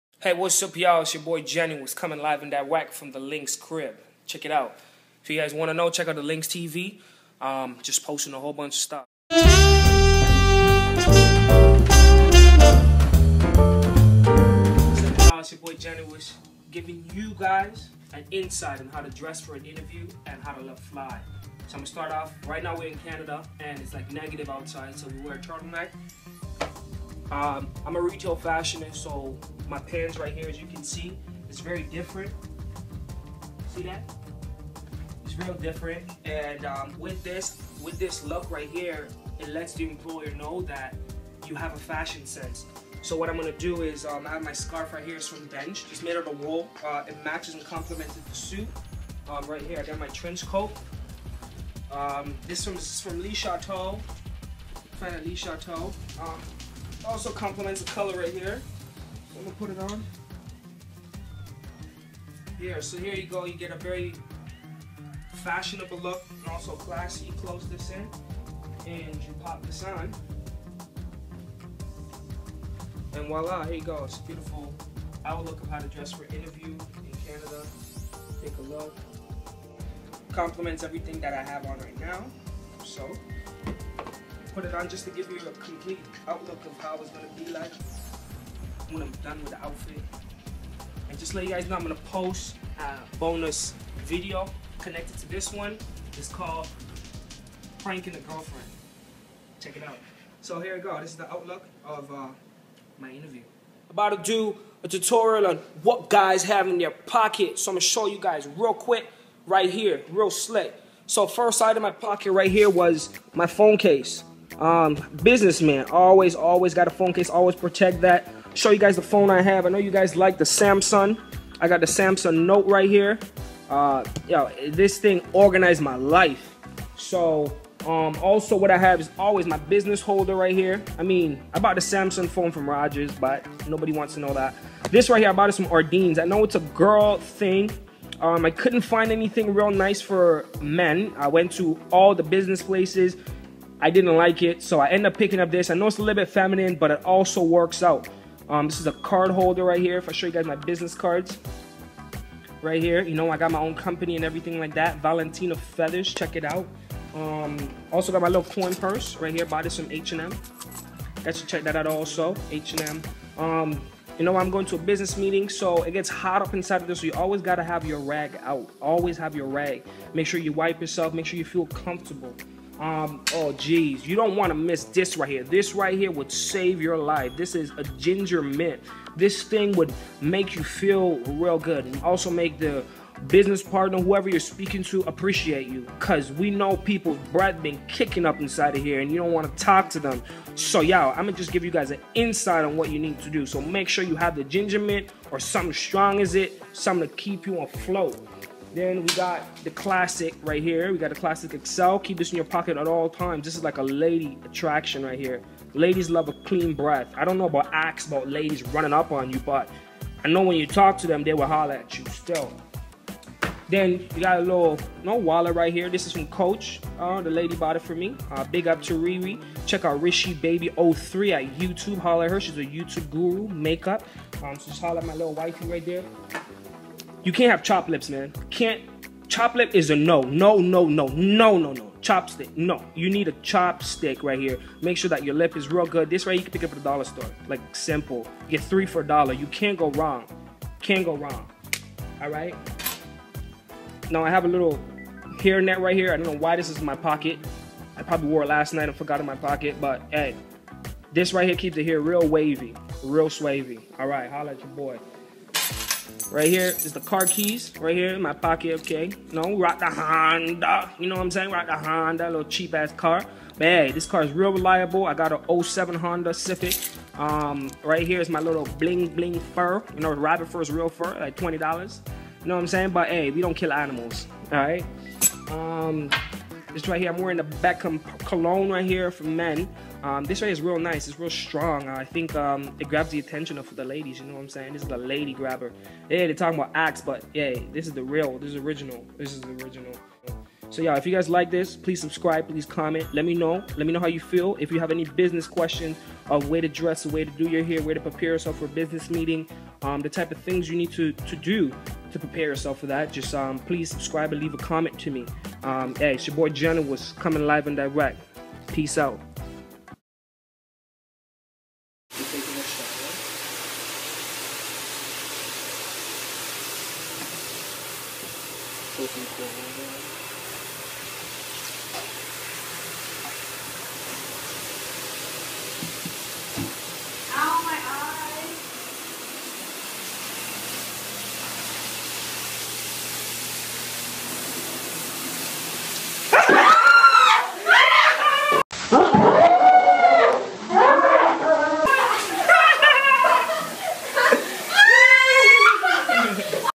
Interview OOTD & What Men Carry In Their Pockets + BF PRANKS GF!
I put this outfit together for an interview occasion.
Turtle Neck - Don't remember
Coat - Le Chateau
Pants - Marshall's
Shoes - Local Shoe Store